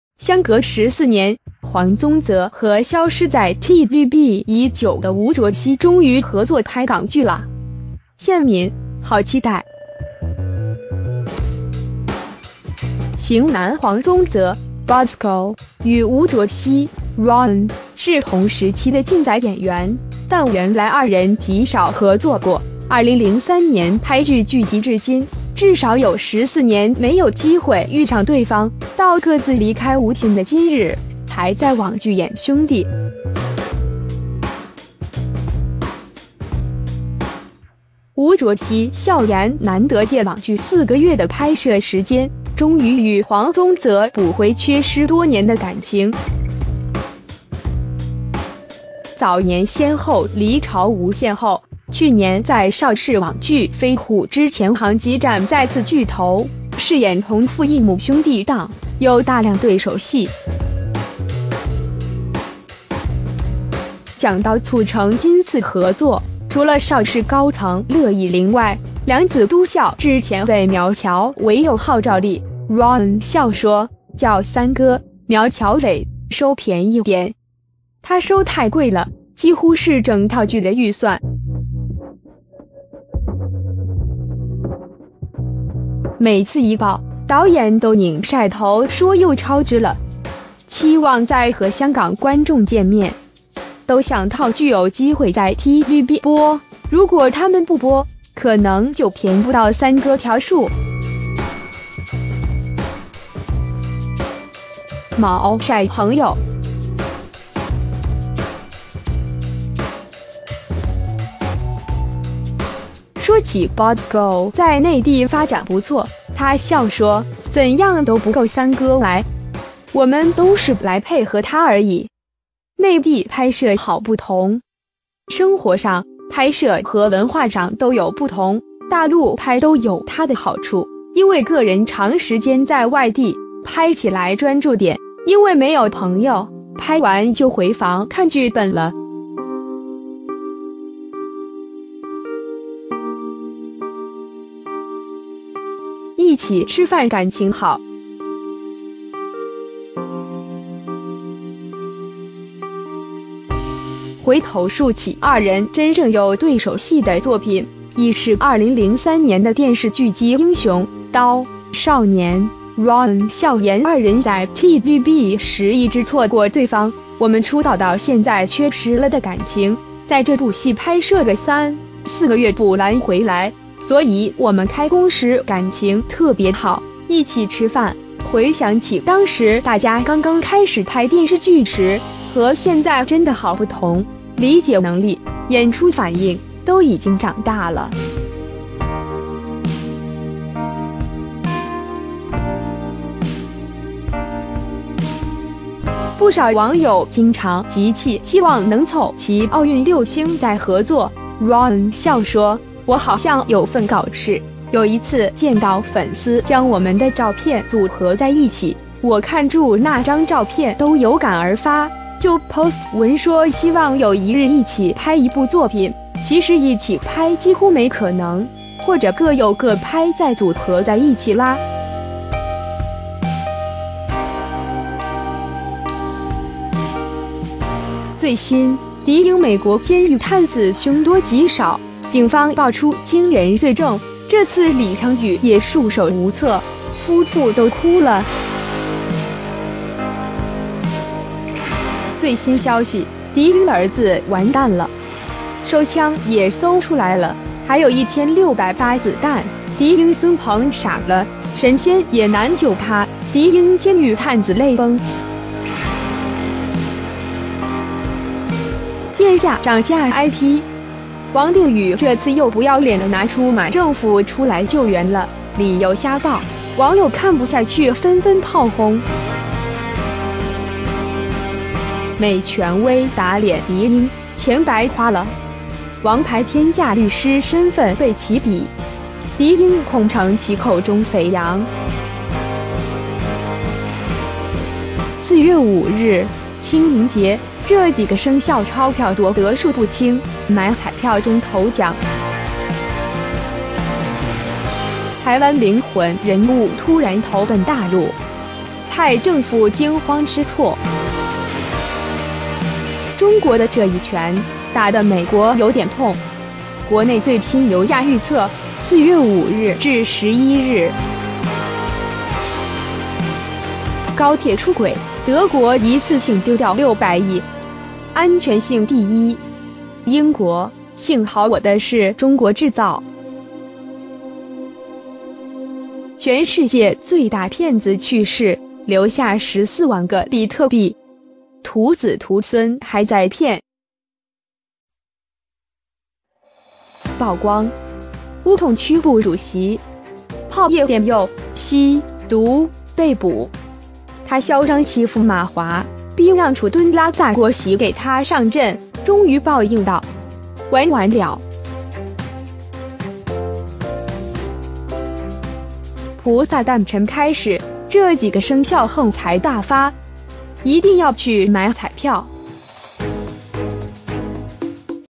相隔14年, 黃宗澤和消失在TVB已久的吳卓羲終於合作拍港劇啦! 線民: 好期待
型男黃宗澤（Bosco）與吳卓羲（Ron）是同時期的靚仔演員，但原來二人極少合作過，2003年拍劇劇集至今，至少有14年沒有機會遇上對方，到各自離開無線的今日，才在網劇演兄弟。吳卓羲笑言難得借網劇四個月的拍攝時間，終於與黃宗澤補回缺失多年的感情！

早年先後離巢無線後，去年在邵氏網劇《飛虎之潛行極戰》再次聚頭，飾演同父異母兄弟檔，有大量對手戲。講到促成今次合作，除了邵氏高層樂易玲外，兩子都笑指前輩苗僑偉有號召力，Ron笑說：「叫三哥（苗僑偉）收便宜點！他收太貴了，幾乎是整套劇的預算。每次一爆，導演都擰曬頭說又超支了！（期望再和香港觀眾見面？）都想套劇有機會在TVB播，如果他們不播，可能就填不到三哥條數。」

冇曬朋友

說起Bosco在內地發展不錯，他笑說：「怎樣都不夠三哥來！我們都是來配合他而已！（內地拍攝好不同？）生活上、拍攝和文化上都有不同，大陸拍都有他的好處，因為個人長時間在外地，拍起來專注點，因為沒有朋友，拍完就回房看劇本了。

一起吃飯感情好

回頭數起二人真正有對手戲的作品，已是2003年的電視劇集《英雄·刀·少年